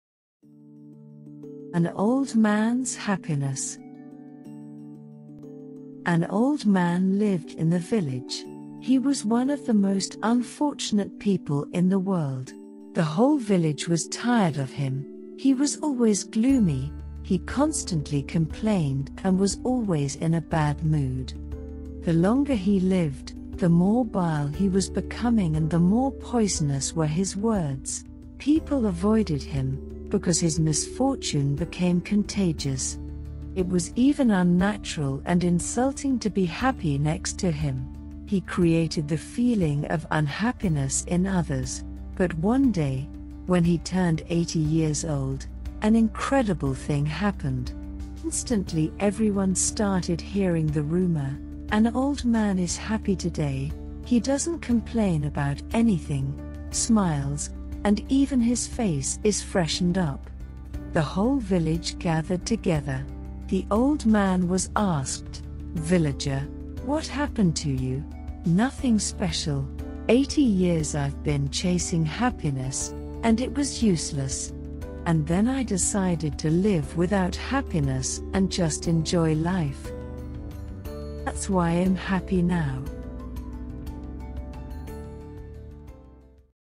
An Old Man's Happiness.Motivational story#happiness
An Old Man's Happiness-motivational story
 An old man lived in the village. He was one of the most unfortunate people in the world. The whole village was tired of him; he was always gloomy, he constantly complained and was always in a bad mood. The longer he lived, the more bile he was becoming and the more poisonous were his words. People avoided him, because his misfortune became contagious. It was even unnatural and insulting to be happy next to him. He created the feeling of unhappiness in others. But one day, when he turned eighty years old, an incredible thing happened. Instantly everyone started hearing the rumour: An Old Man is happy today, he doesnt complain about anything, smiles, and even his face is freshened up. The whole village gathered together. The old man was asked: Villager: What happened to you? Nothing special. Eighty years Ive been chasing happiness, and it was useless. And then I decided to live without happiness and just enjoy life. Thats why Im happy now.

So true.